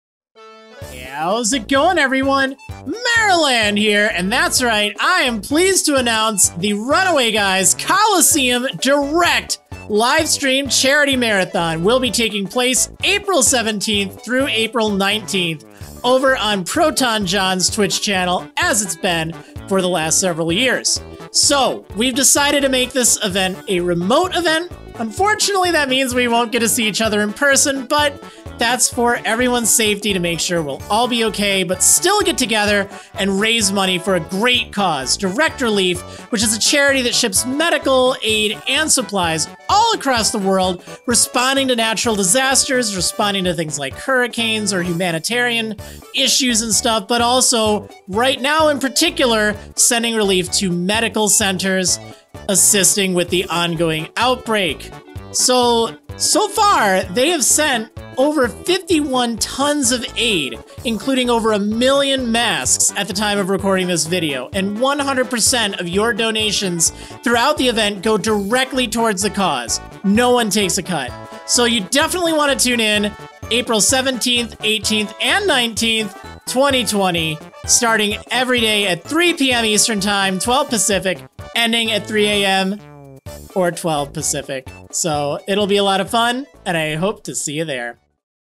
The Runaway Guys Colosseum DIRECT, April 17–19 • Online Charity Marathon benefiting Direct Relief
How's it goin', everyone? Marriland here, and I'm pleased to announce that The Runaway Guys Colosseum DIRECT will be taking place online from April 17–19, 2020, to raise money for Direct Relief, a fantastic charity organization that has been helping out substantially during these troubling times.

As with last year, we're getting together a group of excellent content creators — people that I don't just consider my friends, but my family at this point — to play 36 hours of video games across three days, 12 hours a day. However, unlike last year, for everyone's safety, we're getting together remotely, so things might be a bit different this year but I hope they will still be a lot of fun and raise money for a good cause!

You can tune into the stream on Proton Jon's Twitch channel from April 17–19, 2020 at 3pm – 3am EDT (12pm – 12am PDT) each day.

You can also keep up with information about the event by following Direct Relief Gaming's Twitter: